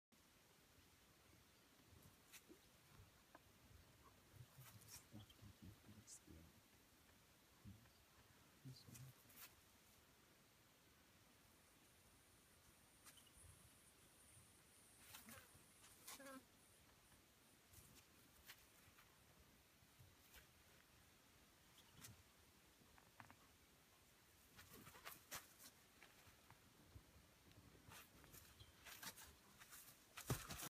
Curious Rhino Calf at Lion Sands
The curious, yet friendly Rhino Calf was filmed by Lion Sands guest, Michael Ricci